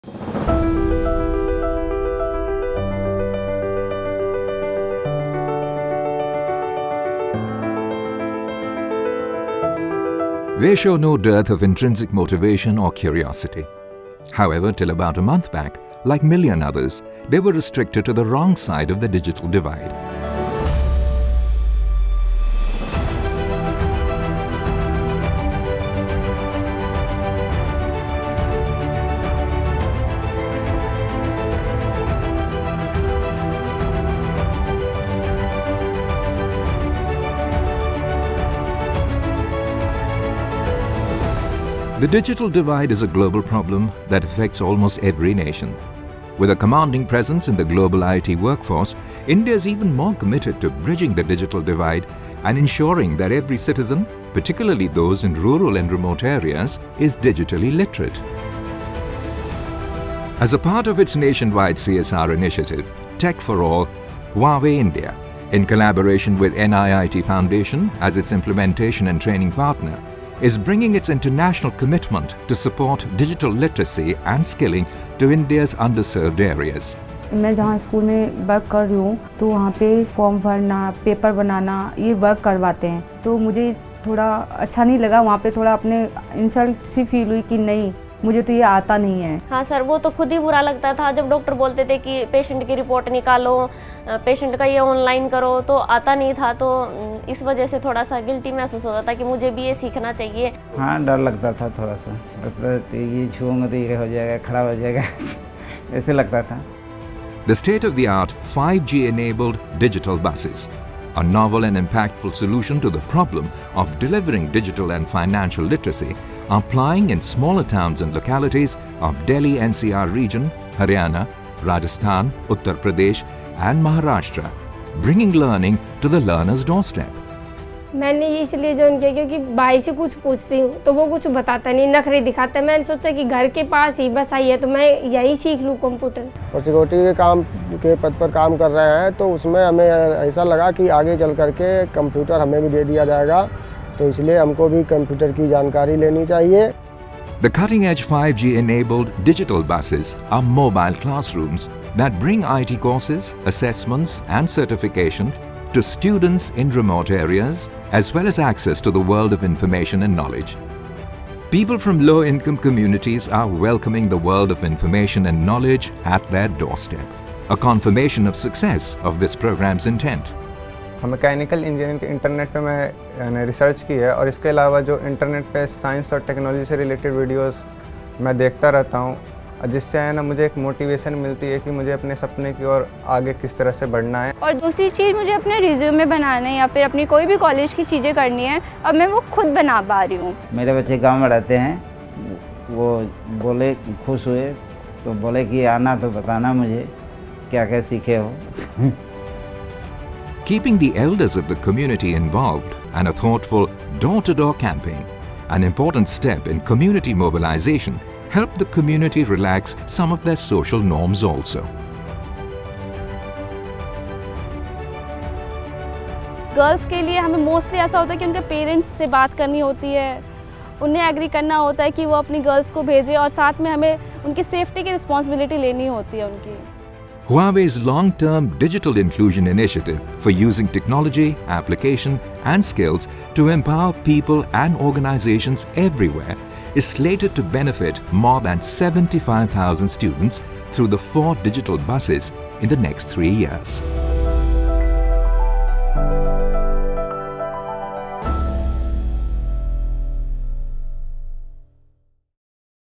Digital Literacy with a mobile Digital Classroom | Huawei Digital Bus | Computer Training
In September 2022, Huawei India and NIIT Foundation began discussing and collaborating on how to solve the problem of digital divide and digital illiteracy in remote areas of India. NIIT Foundation has already successfully been running multiple digital vans that take education to the doorstep of individuals in remote or isolated areas. Tapping into NIIT Foundation's expertise regarding digital education and upskilling, Huawei India CSR joined hands with NF to support the Prime Minister's mission Antyodaya program to spread digital literacy all over India. India has been actively contributing to the global Information Technology sector and it was imperative that we skill and educate more citizens to help strengthen the sector.
The Huawei - NIIT Foundation Digital Bus was flagged off on 16th January, 2023 in New Delhi. The 5G enabled, solar powered mobile digital classrooms  are outfitted with 16desktop computers, 30 mobile phones, inside and outdoor digital screens. The DigiBus will offer digital awareness, education, and skilling courses to youth, women, and the impoverished. The Digital Bus uses cutting-edge technology to continuously deliver top-notch educational facilities to remote communities, including 5G Communications, Immersive & Virtual Collaboration, Content Management, Security, and Edge AI.We hope to reach 75,000 beneficiaries in the first phase of this project which will be implemented in Delhi-NCR, Rajasthan, Maharashtra, Uttar Pradesh and Haryana.
There has been a very positive impact that the bus has had in the past 7 weeks since its launch. Multiple beneficiaries have shared that the education they received has helped them perform better at work, help their children with their studies, become self-sufficient as well as eliminate the fear of technology. It has given citizens respect and has helped relax some orthodox societal norms, bringing about community social development.

Take a tour of the Digital Bus with us and let's collectively dream of developing empowered and aware communities and citizens.